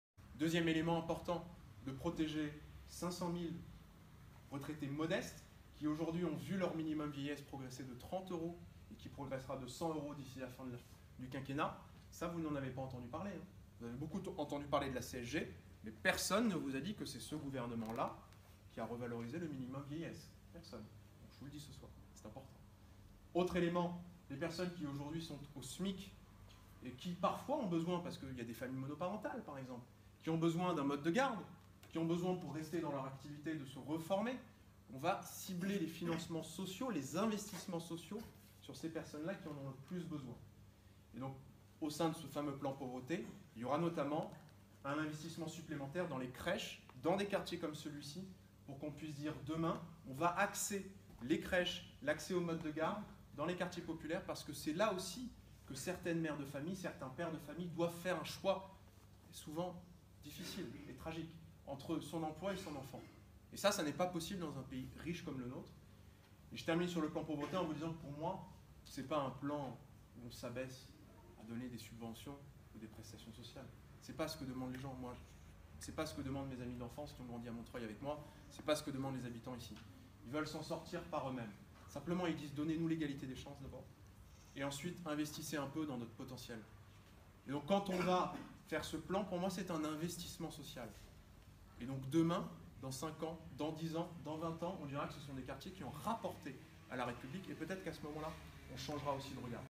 Réunion publique de Laurence Maillart (LREM) et Rayan Nezzar à Rennes (2/2)
"L'objectif du plan pauvreté ? Cibler les investissements sociaux sur celles et ceux qui en ont le plus besoin"

Les échanges ont porté sur la préparation du plan pauvreté et ses principaux objectifs : l'approche doit prévenir durablement la reproduction des inégalités